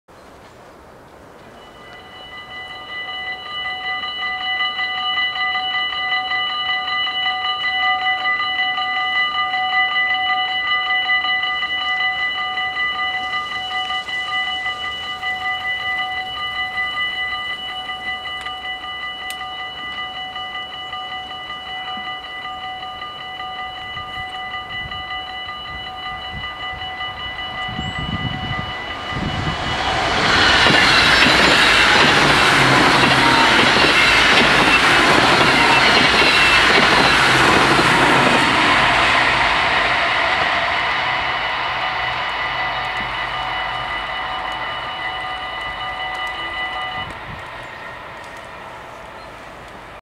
Spoorwegovergang Assel // Dutch railroad crossing
Locatie: Alverschotenseweg
Traject: Apeldoorn - Amersfoort
Soort: AHOB
Rode lichten: 10
Oranje pijl: 1
Bellen: 4
Bomen: 2
Andreaskruisen: 2
Passeren:
- ICMm als IC Enschede → Schiphol